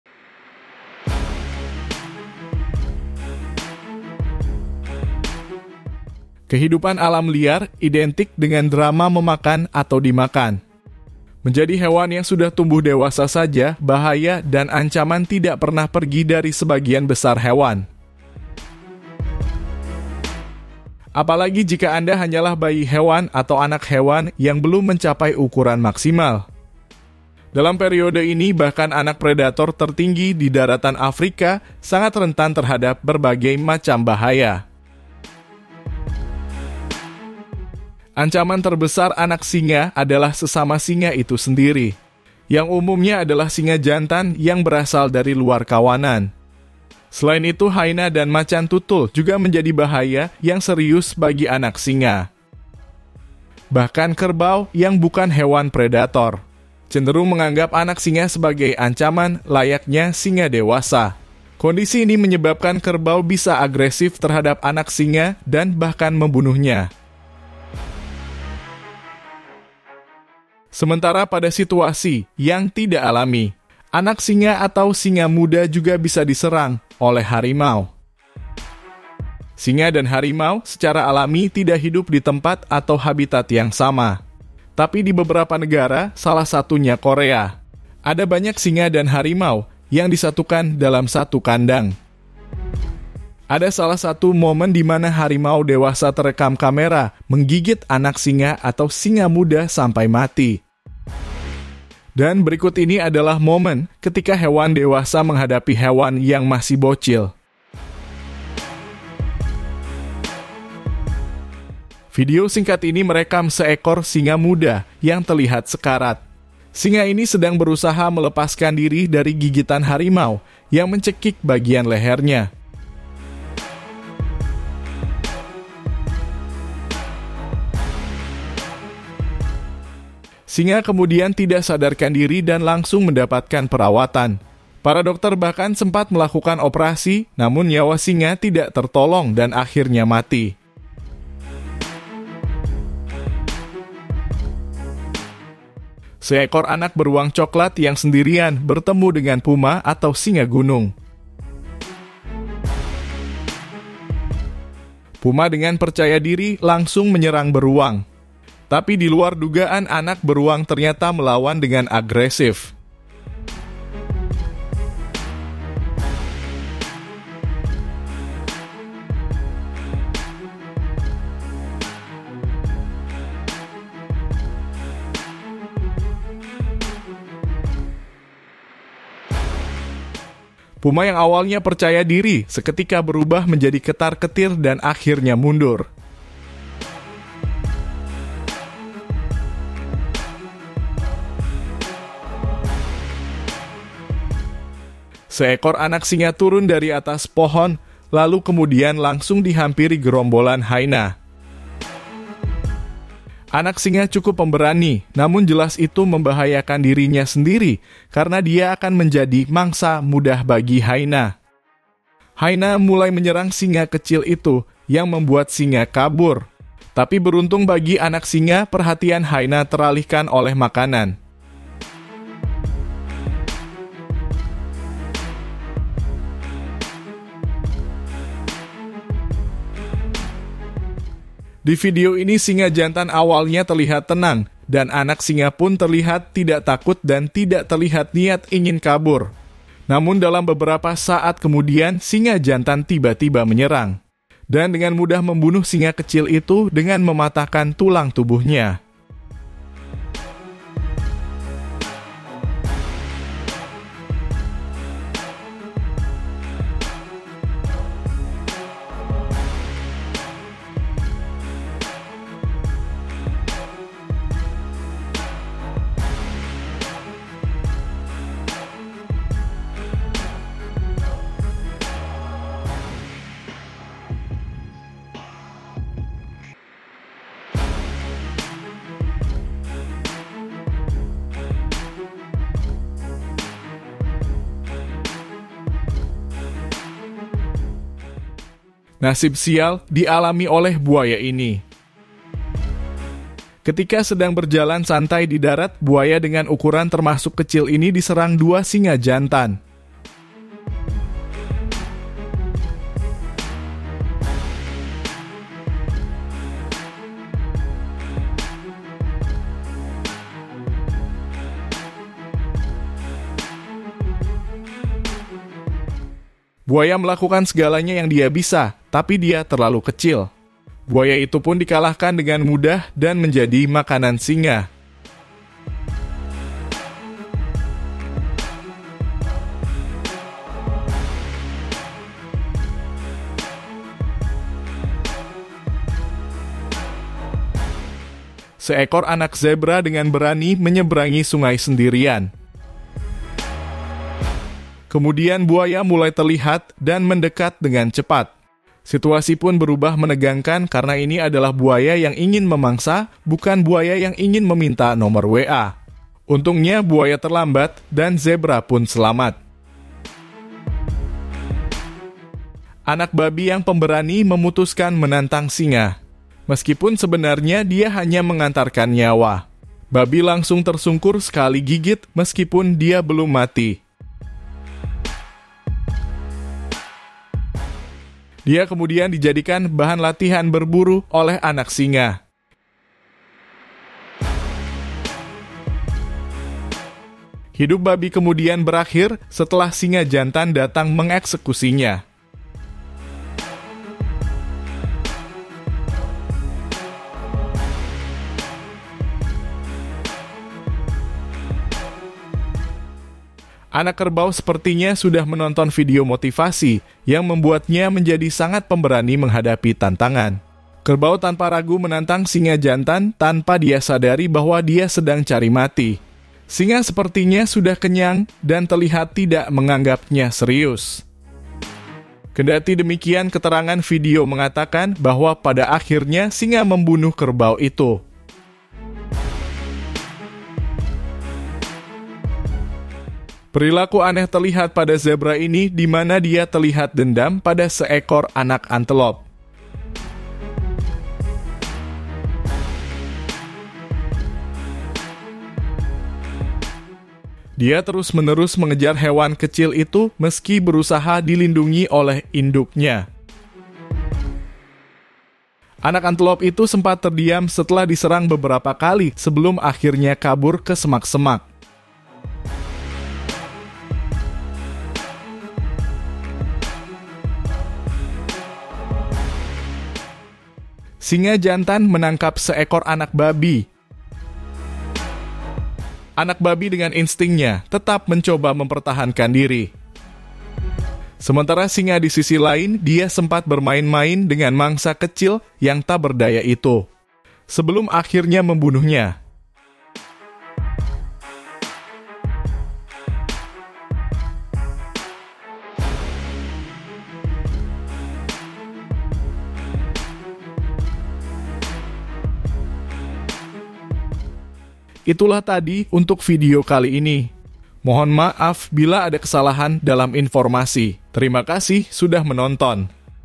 TOP Momen Anak Hewan Menghadapi Hewan besar
TOP Momen Anak Hewan Menghadapi Hewan.
membahas momen pertarungan hewan buas di alam liar atau pertarungan hewan buas di alam liar terbaik atau pertarungan hewan di alam liar paling seru.